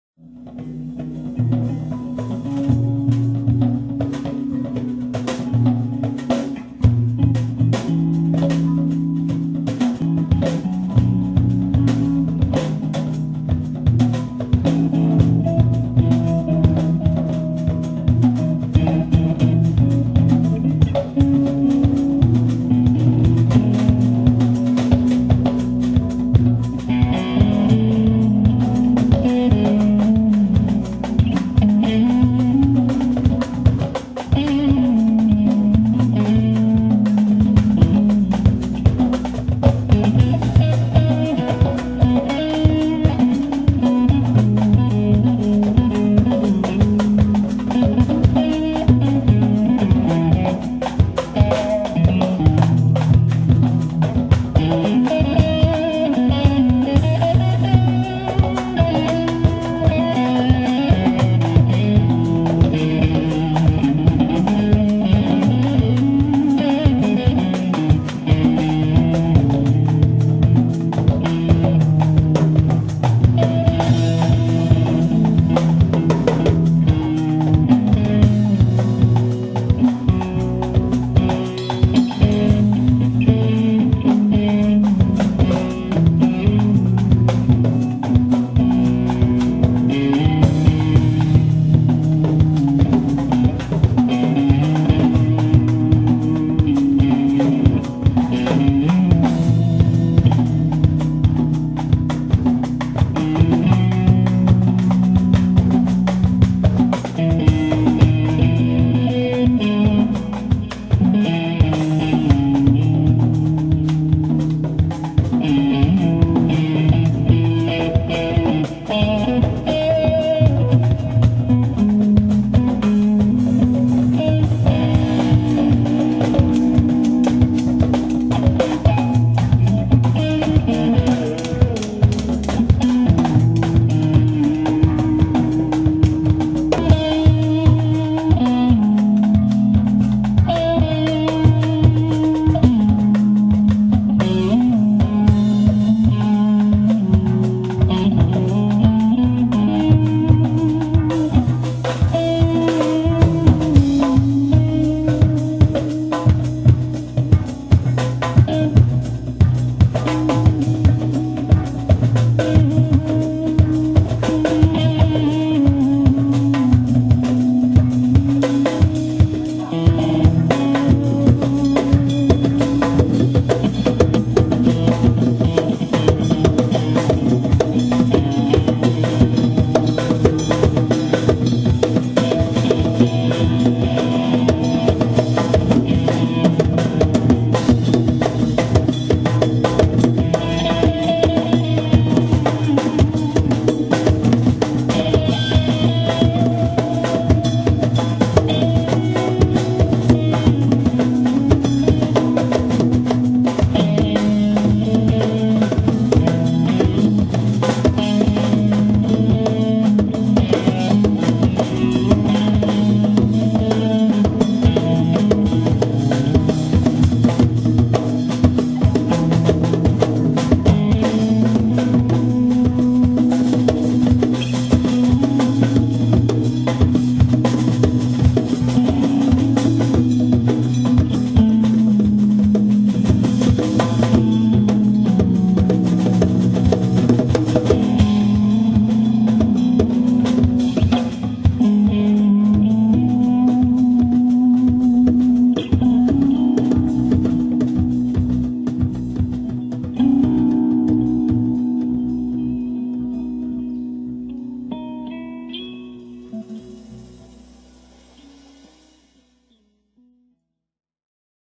On the Road with Thriloka - Dusk Jam
Captured Live in Kandalama, Sri Lanka. The band started jamming this after the sound check was done. It took me awhile to realize that there was something happening between the guys so the camera rolled late into the jam (sorry). Afterwards I went to them like an idiot and said: 'can you play it again this evening?'